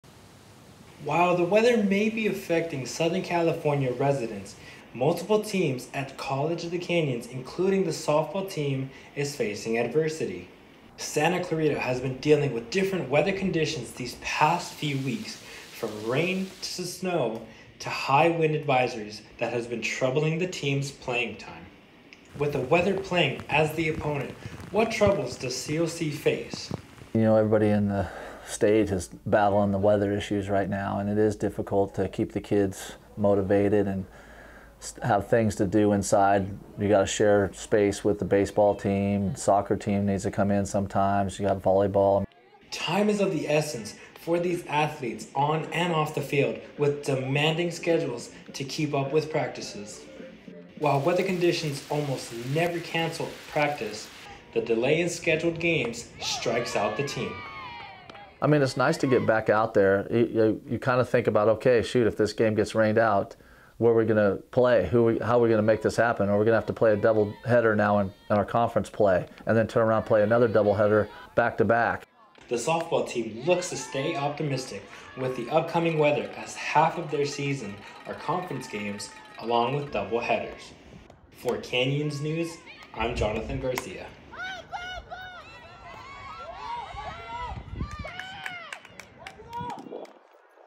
Softball Weather
While the weather is affecting southern California residents, multiple teams at College of the Canyons, including the Softball team, is facing adversity. Santa Clarita has been dealing with different weather conditions these past few weeks from rain to snow, to high wind advisories that has been troubling the team’s playing time.